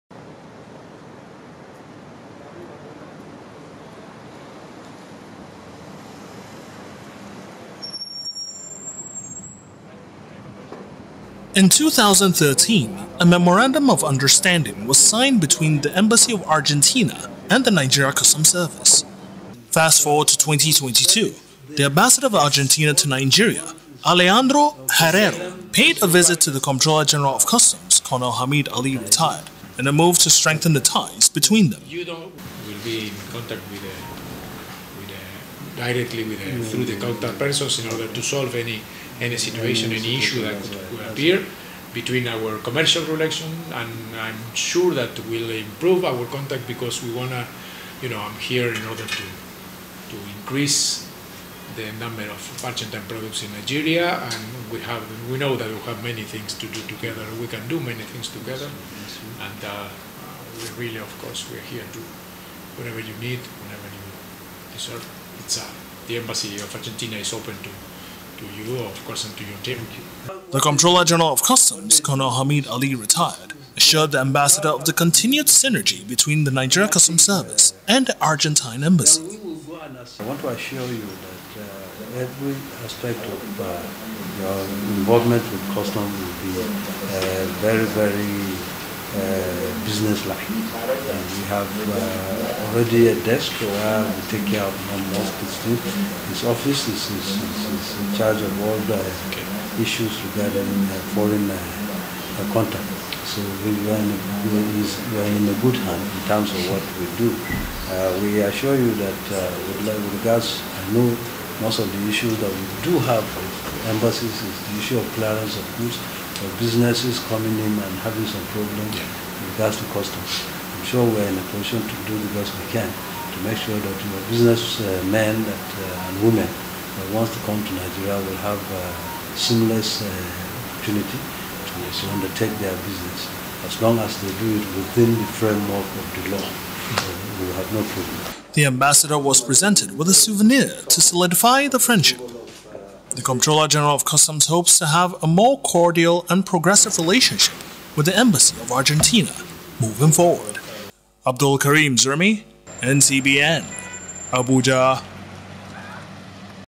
Argentinian Ambassador Visits CGC, Seeks to Improve Relationship with Customs
- Courtesy Visit

BY:
NCBN
Nigeria Customs Broadcasting Network